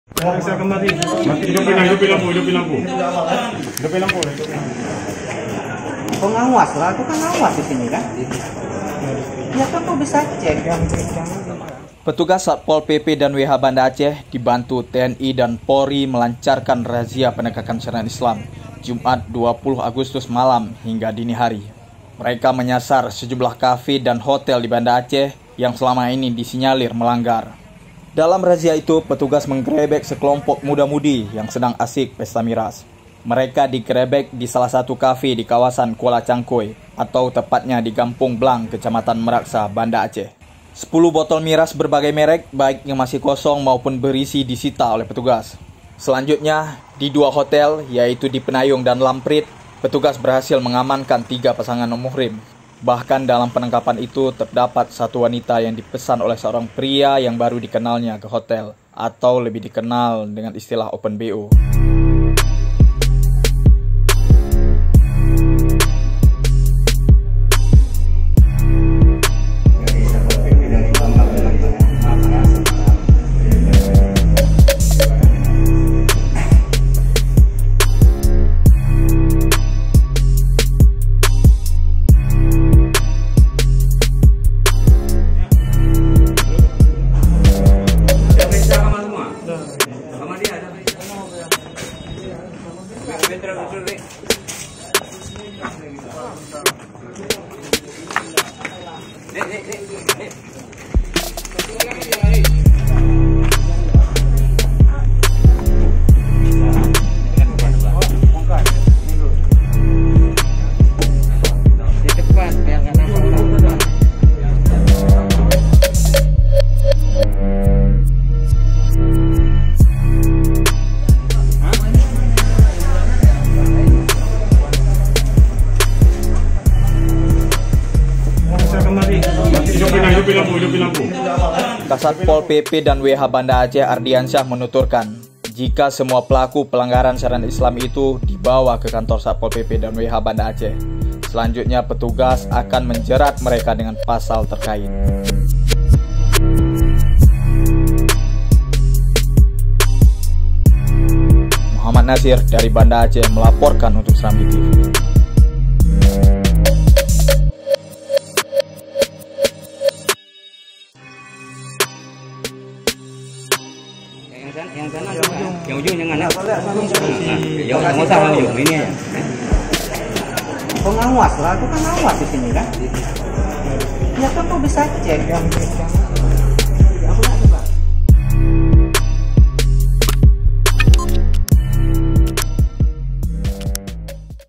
Muda mudi Digerebek saat Pesta Miras di Banda Aceh, Ikut Diciduk Cewek 'Open BO'
Laporan Muhammad Nasir | Banda Aceh

SERAMBINEWS.COM, BANDA ACEH - Petugas Satpol PP dan WH Banda Aceh, dibantu TNI dan Polri melancarkan razia penegakan syariat Islam, Jumat (20/8/2021) malam, hingga Sabtu (21/8/2021) dinihari.

Mereka menyasar sejumlah cafe dan hotel di Banda Aceh yang selama ini disinyalir melanggar syariat Islam.

Dalam razia itu, petugas menggerebek sekelompok muda-mudi yang sedang pesta miras di salah satu cafe di kawasan Kuala Cangkoi, atau tepatnya di Gampong Blang, Kecamatan Meuraxa, Banda Aceh.

Sebanyak 10 botol miras berbagai merk, baik yang masih kosong maupun berisi disita petugas.

Kemudian petugas melanjutkan razia di dua hotel di kawasan Peunayong dan Lampriet. Sehingga berhasil mengamankan 3 pasangan non muhrim.

Bahkan dalam penangkapan itu, terdapat satu wanita yang dipesan oleh pria yang baru dikenalnya ke hotel atau lebih dikenal dengan istilah 'Open BO'.

Kasatpol PP dan WH Banda Aceh, Ardiansyah menuturkan, jika semua pelaku pelanggar syariat itu dibawa ke Kantor Satpol PP dan WH.

Selanjutnya petugas akan menjerat mereka dengan pasal terkait.